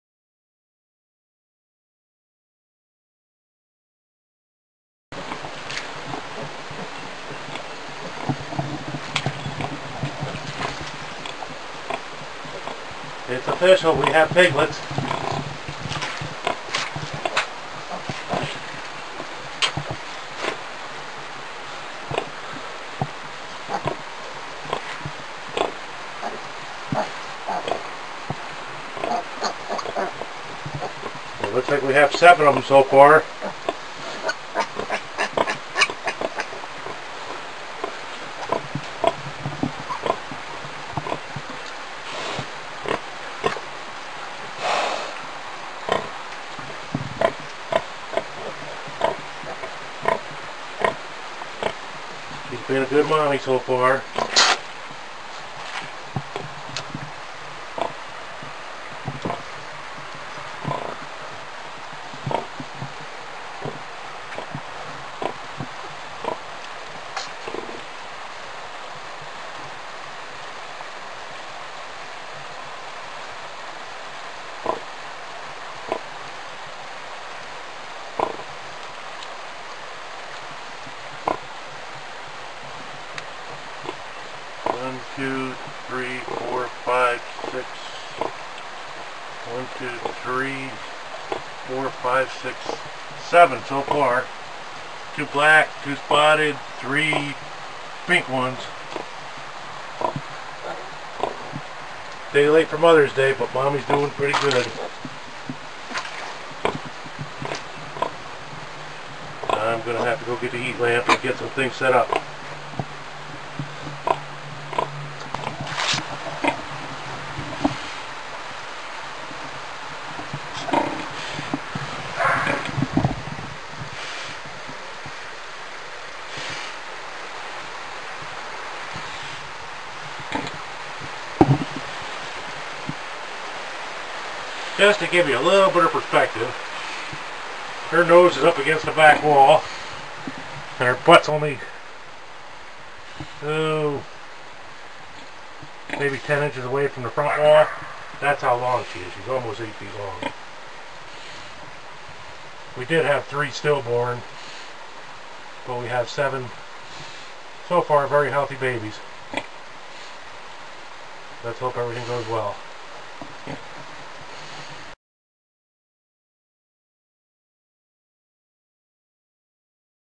FIRST TIME MOM PIGLETS ARE HERE 5/13/2019
We are very happy Hunnie had 7 healthy piglets so far she did have 3 still born but she is being a very good mom we will keep you updated.